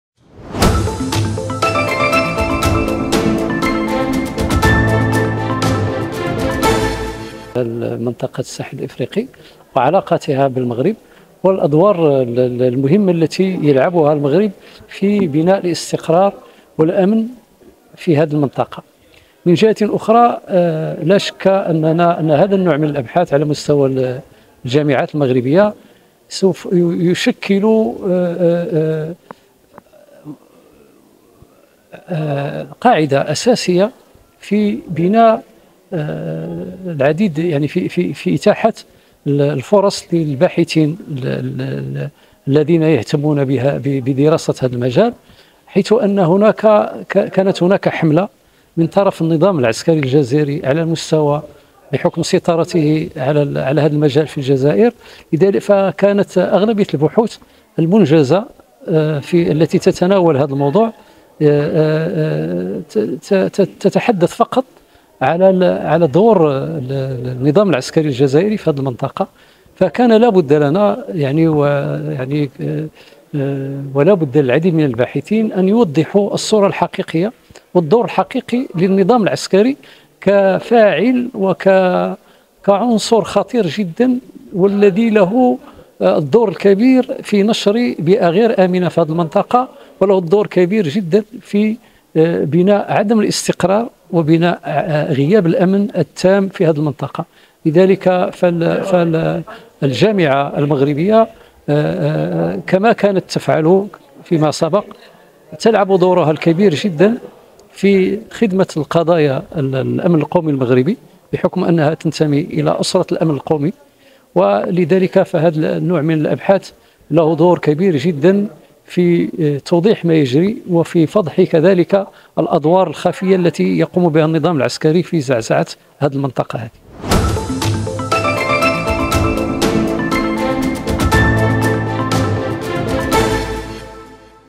هذا ما ينبغي للبحث العلمي كشفه حول دور عسكر الجزائر في نشر بيئة غير آمنة بمنطقة الساحل
يكشف الخبير في الشؤون العسكرية والأمنية محمد الطيار ما الذي ينبغي فغله في البحث العلمي لفضح دور عسكر الجزائر في نشر بيئة غير آمنة بمنطقة الساحل.